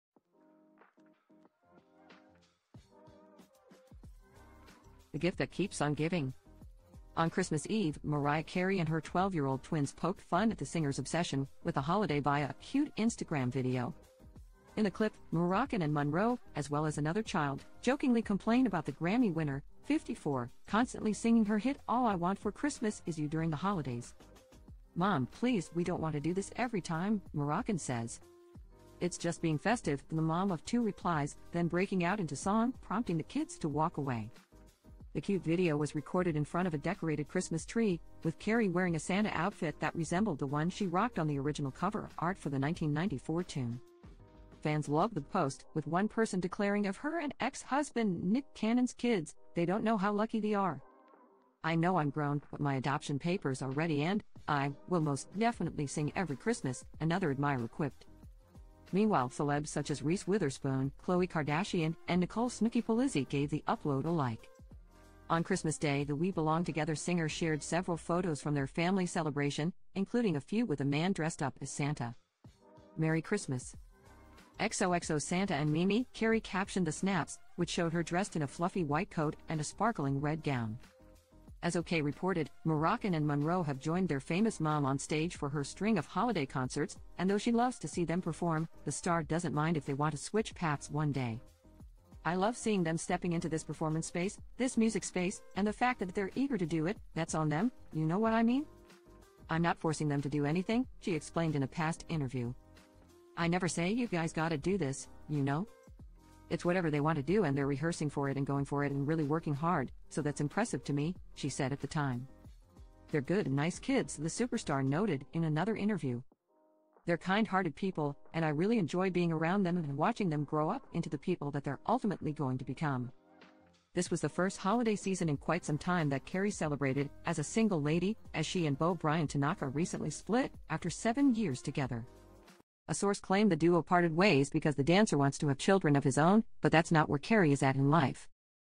Mariah Carey and Her 12-Year-Old Twins Joke About the Singer's Obsession With Christmas in Funny Vid
Mariah Carey and Her 12-Year-Old Twins Joke About the Singer's Obsession With Christmas in Funny Video: Watch